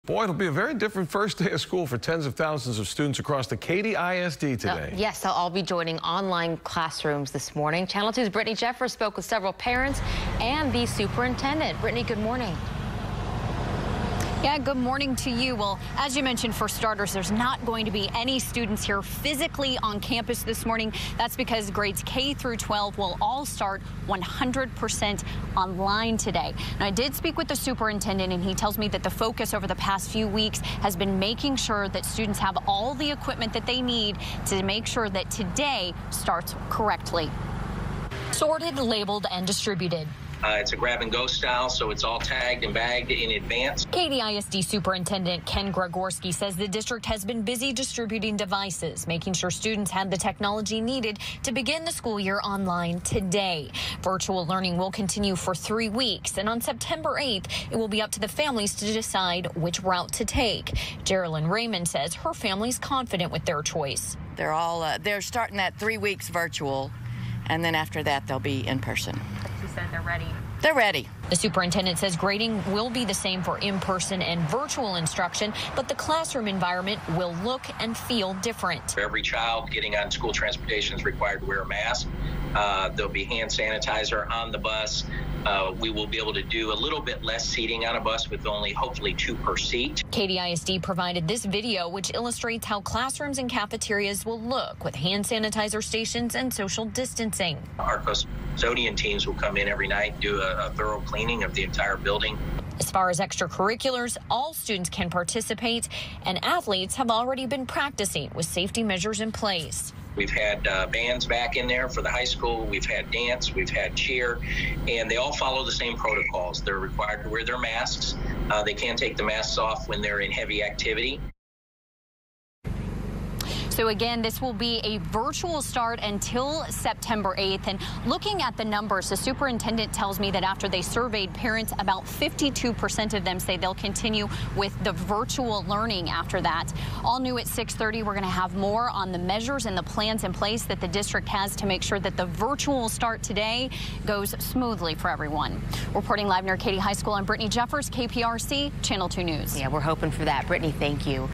Katy ISD students begin school year online
Katy ISD students begin school year online Wednesday.